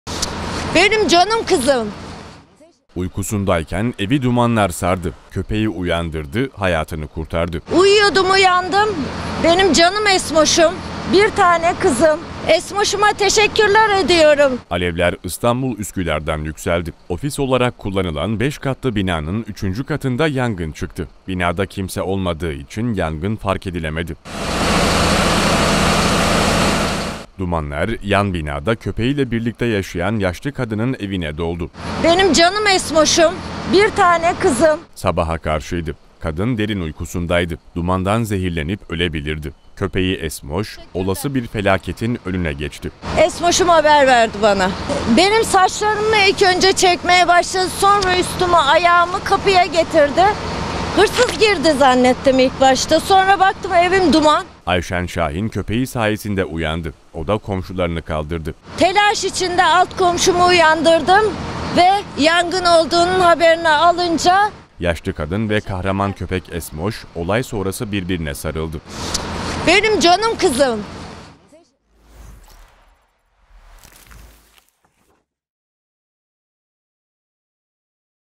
Kahraman köpek Esmoş, sahibini kurtardı
Üsküdar'da ofis olarak kullanılan beş katlı binanın üçüncü katında yangın çıktı. Binada kimse olmadığı için yangın fark edilmezken, dumanlar yan binada köpeği ile birlikte yaşayan Ayşen Şahin'in evine doldu.

Bu sırada uyuyan Ayşen Şahin’i, köpeği Esmoş önce saçlarından ardından ayağından çekerek uyandırmak istedi. Köpeğinin ısrarlı hareketleri sonucu uyanan Şahin, evin içindeki dumanı görünce panik halinde alt kattaki komşusunu uyandırdı.

Komşusunun ihbarı üzerine olay yerine itfaiye, sağlık ve polis ekipleri sevk edildi. İtfaiye ekipleri, merdiven yardımı ile pencereden binaya girerek yangına müdahale etti. Yangın kısa sürede söndürüldü. Yangınla ilgili inceleme başlatıldı.

TÜRKSAT 4A
Frekans: 12729
Sembol: 30000
Polarizasyon: V (Dikey)